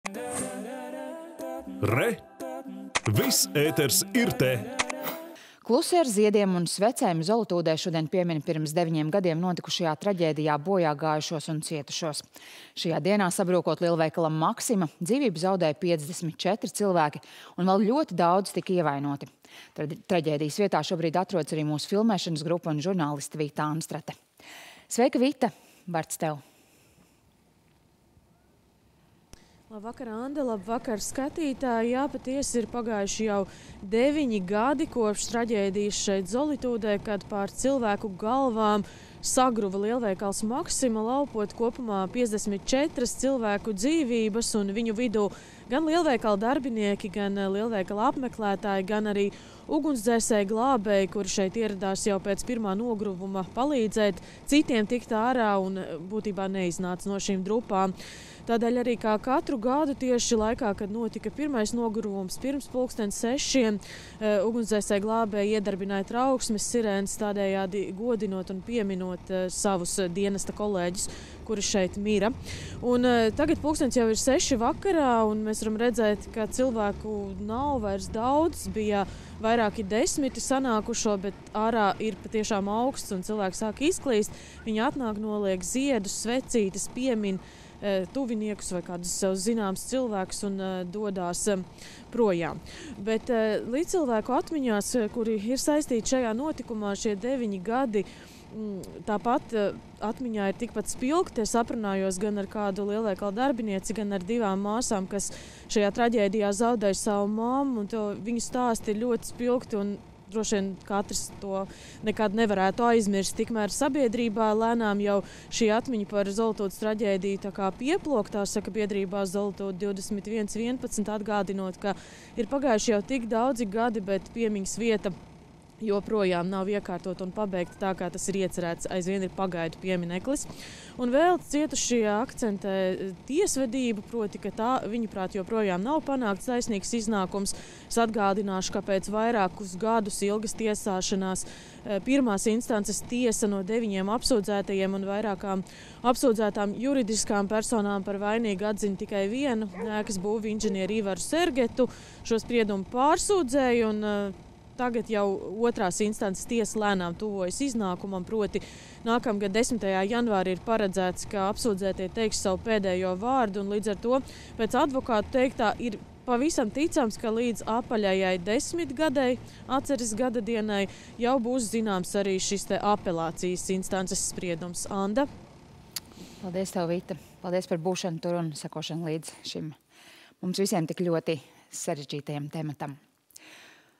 Aprit deviņi gadi kopš Zolitūdes traģēdijas
Klusi, ar ziediem un svecēm Zolitūdē šodien piemin pirms deviņiem gadiem notikušajā traģēdijā bojāgājušos un cietušos. Šajā dienā, sabrūkot lielveikalam “Maxima” dzīvību zaudēja 54 cilvēki un vēl ļoti daudzi tika ievainoti. Traģēdijas vietā šobrīd atrodas arī mūsu filmēšanas grupa un žurnāliste Vita Anstrate.